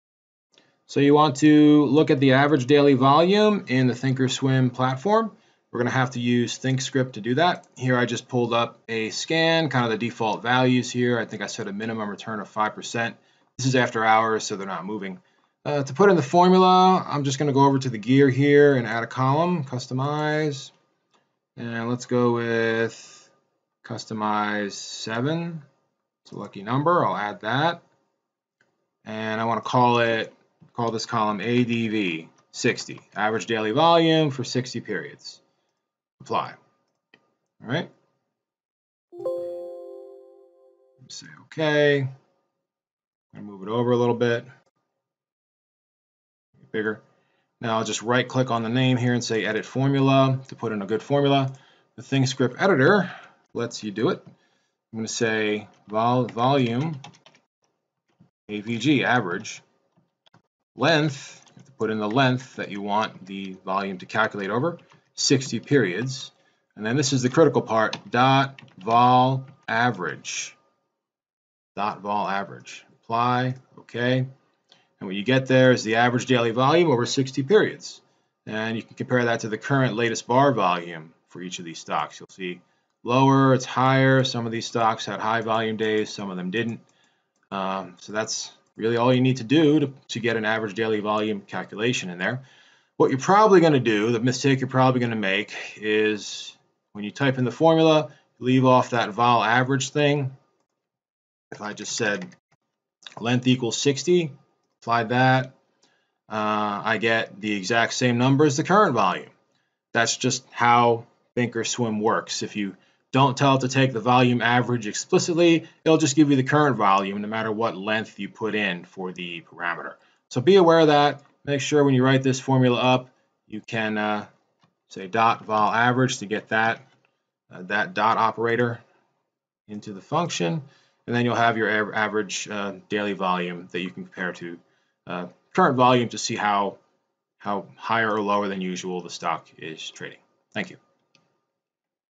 Average Daily Volume - Think Or Swim
No BS. Just the code.

Calculate average daily volume in Thinkscript.